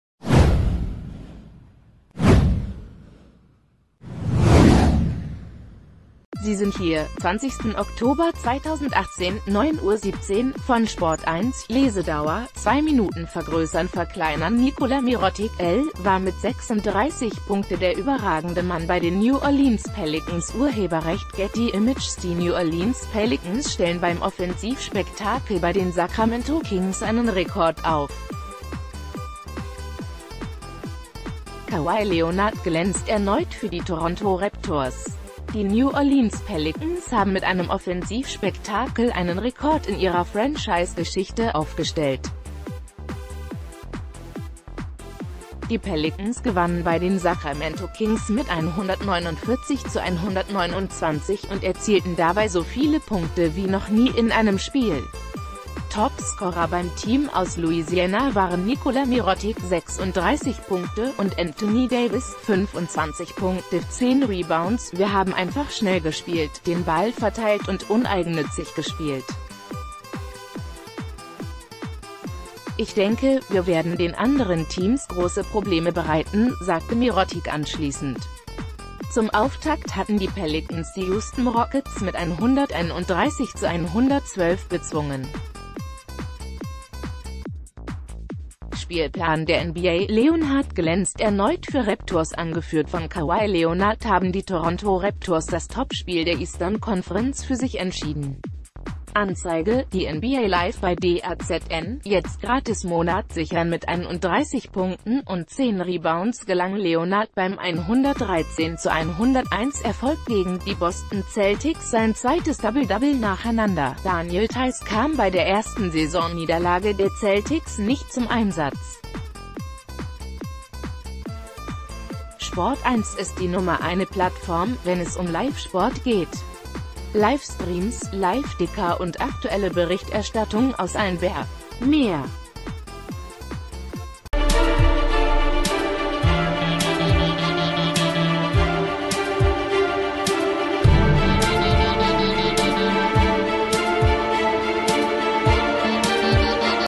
NBA: Rekordsieg für New Orleans Pelicans - Leonard glänzt bei Raptors-Sieg
Sie sind hier: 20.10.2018 / 09:17 Uhr - von SPORT1 / Lesedauer: 2 Minuten vergrößernverkleinern Nikola Mirotic (l.) war mit 36 Punkte der überragende Mann bei den New Orleans Pelicans © Getty Images Die New Orleans Pelicans stellen beim Offensivspektakel bei den Sacramento Kings einen Rekord auf. Kawhi Leonard glänzt erneut für die Toronto Raptors. Die New Orleans Pelicans haben mit einem Offensivspektakel einen Rekord in ihrer Franchise-Geschichte aufgestellt. Die Pelicans gewannen bei den Sacr...